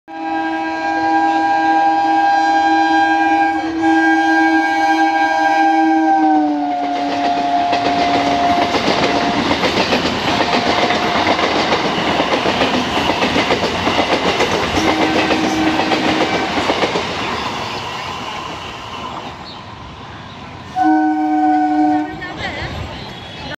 Shantipur Galloping local in Bombardier Avatar ! - Indian Railways
31531 Sealdah - Shantipur Galloping local skipping Belgharia in a fine speed !